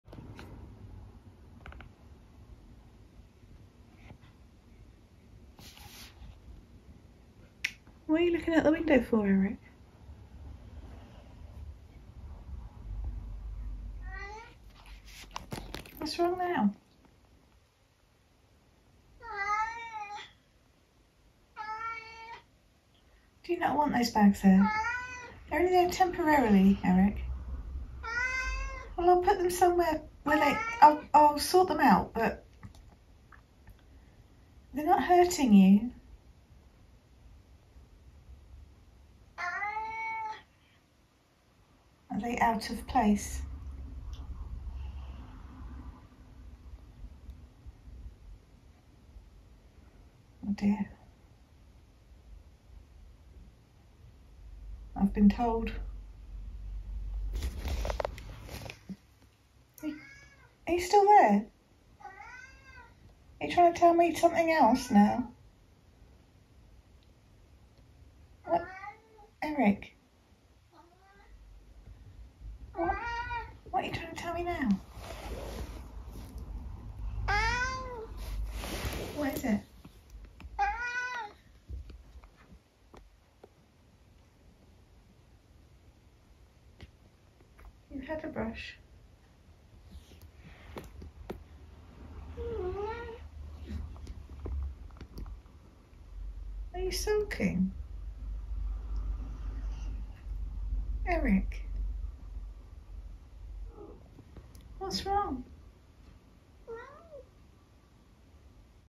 Eric tells me straight
Eric is a very vocal cat, he always has something to say about every situation.

Eric is a 12 year old tabby cat from the uk.